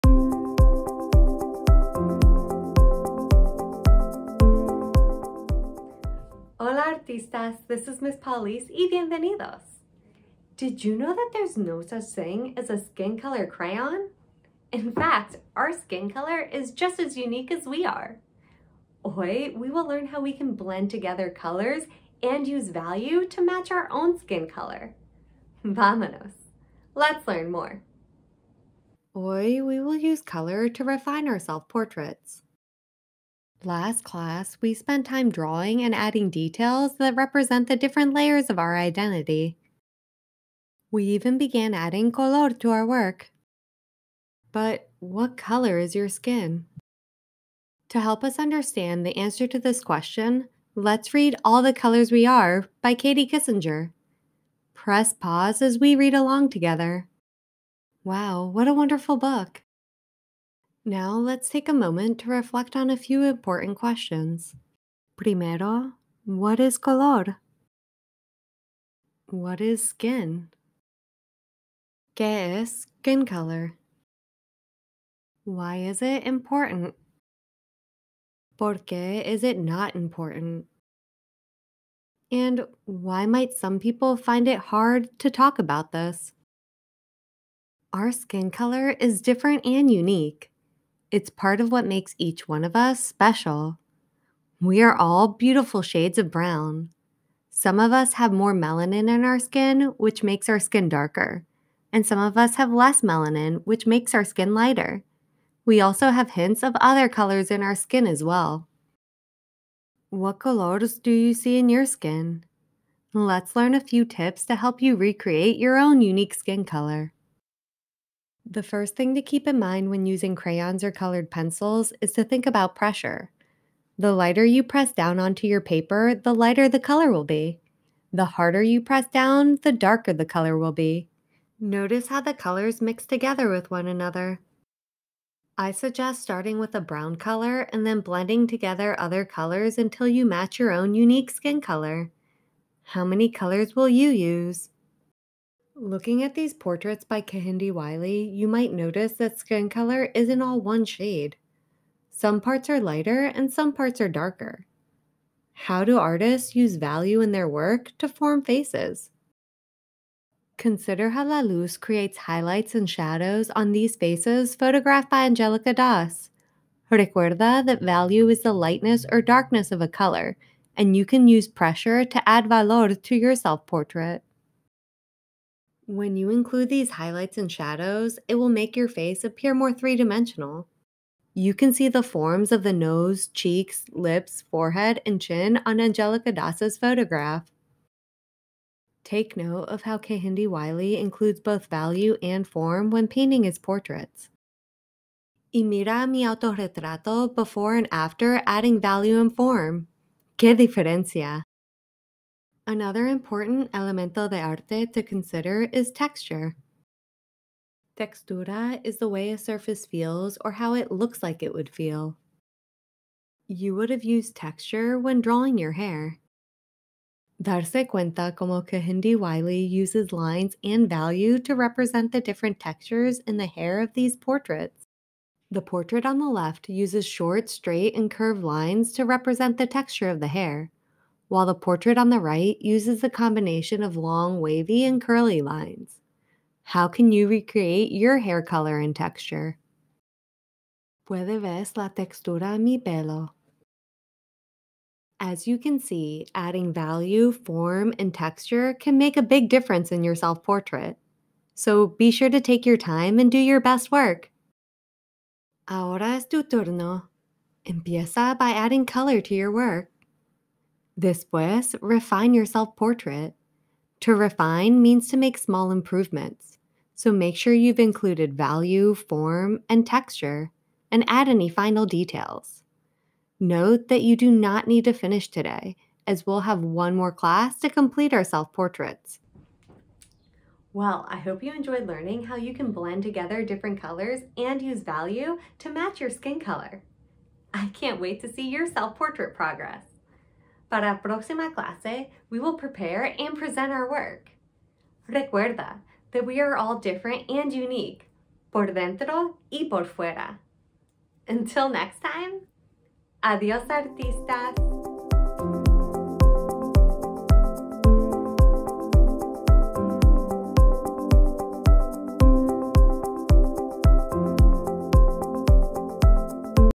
Self-Portrait Drawings: Part 4 (Refining Our Work)
Hola artistas, this is Ms. Paula Liz y bienvenidos! Did you know that there is no such thing as a “skin” color crayon? In fact, our skin color is just as unique as we are. Hoy we will learn how we can blend together different colors and use value to match our own skin color. Vamanos, let’s learn more!

EDUCATORS! To download this lesson and presentation, please visit my TpT

In part four students will begin to add color, texture, value, and form to their self-portraits. This begins with a conversation about skin color (Recommended Read Aloud: ‘All the Colors We Are’ by Katie Kissinger). They will then learn how they can blend together different colors and use value to match their own skin color. In my example I use crayons, but colored pencils, markers, or paint can be used as well.

Featured Artists Include: Kehinde Wiley, Angelica Dass, and Byron Kim
Recommended Read Aloud: ‘All the Colors We Are’ by Katie Kissinger